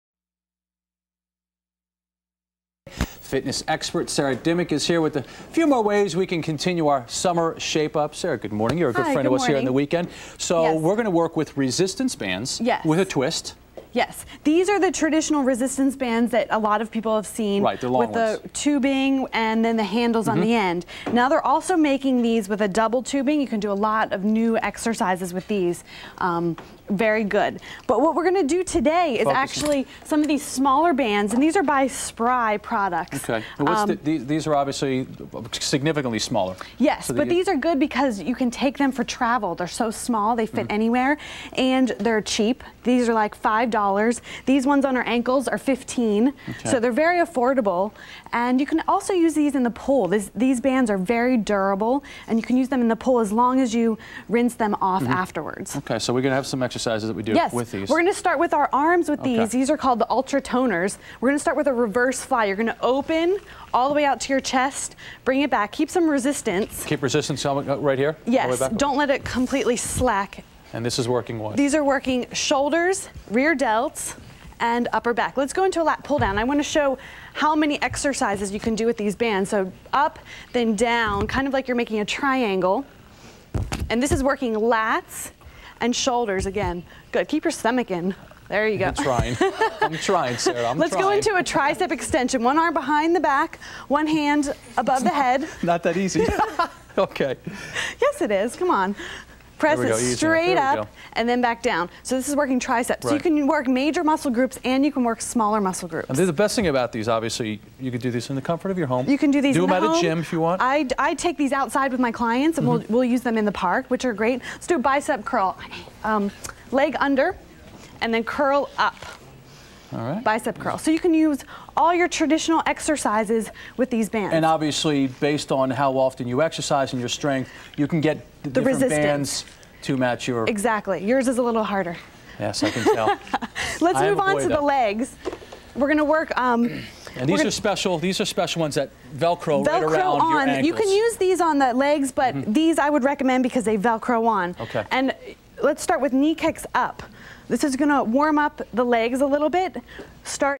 Summer Shape Up with Easy to Use Exercise Bands
Get fit and toned with easy to use exercise bands from Spri Products.  These are great for travel or for exercising outdoors or in small spaces.